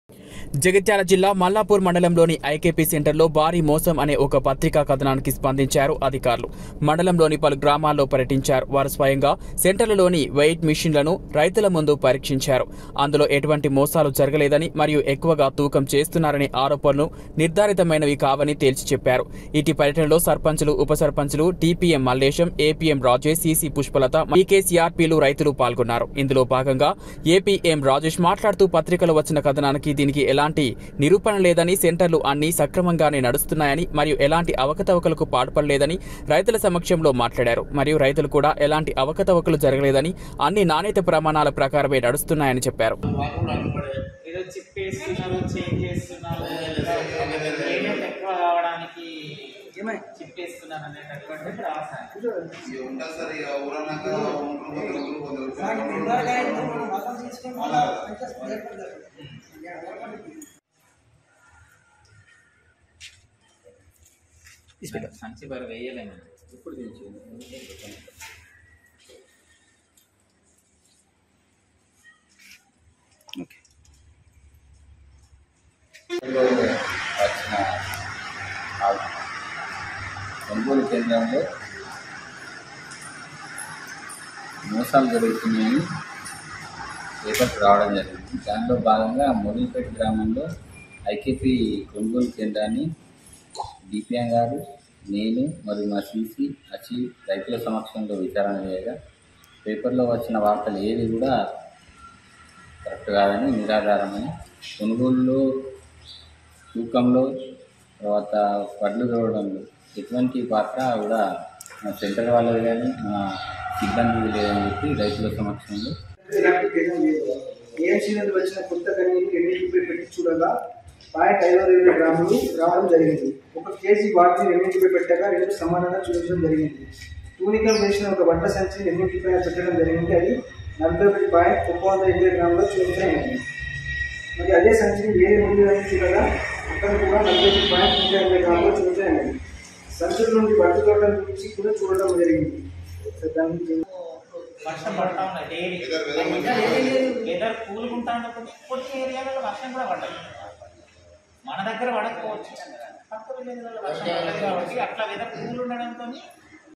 మల్లాపూర్ మండలంలోని ఐకెపి సెంటర్లలో భారీ మోసం అనే ఒక పత్రిక కథనానికి
Authorities responded to a magazine article alleging massive fraud at IKP centers in the Mallapur zone of Jagittala district. He visited several villages in the zone and personally inspected the weight machines at the center in front of the farmers and concluded that there were no scams and the allegations of overweight were not substantiated.